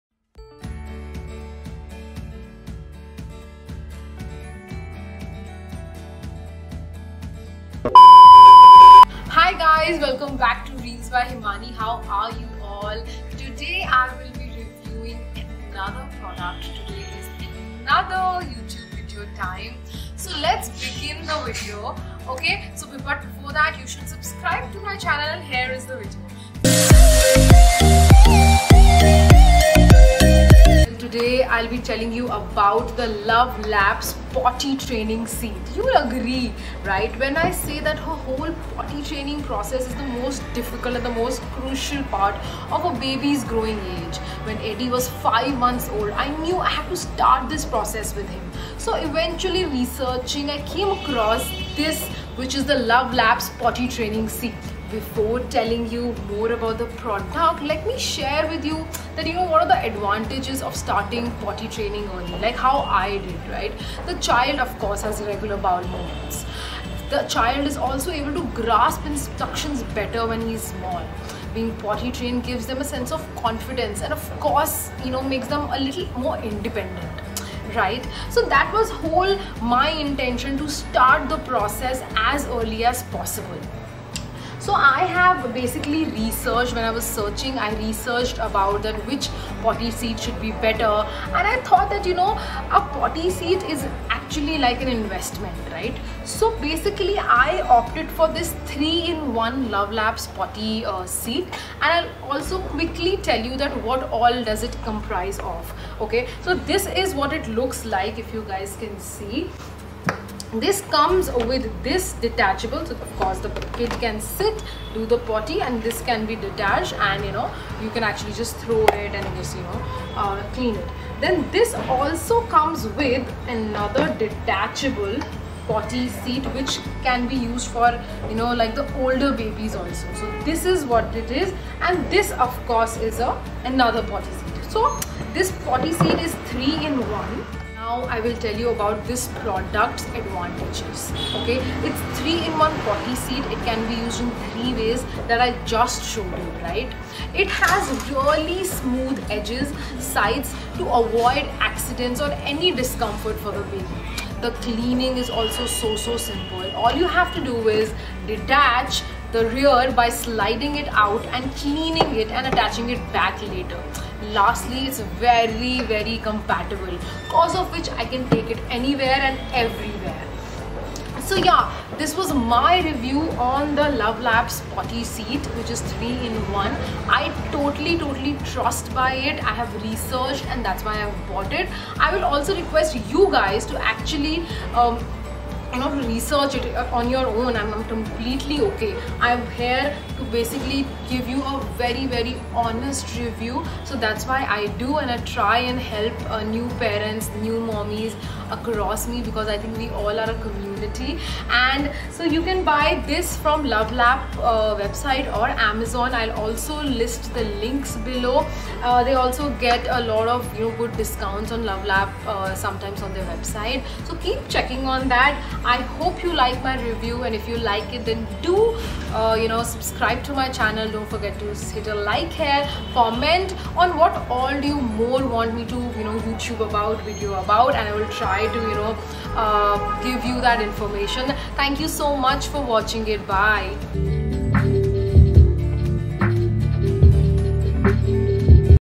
How I aced potty training my child | Luvlap | Review
Potty training is a crucial yet the most difficult part in the entire parenting journey.

While its a headache of a task here’s me giving you some tips on how to do the same along with outing a secret of my fav potty seat for my baby Edi✨

Buy my favourite @luvlap-happybabieshappiermoms pottyseat here: her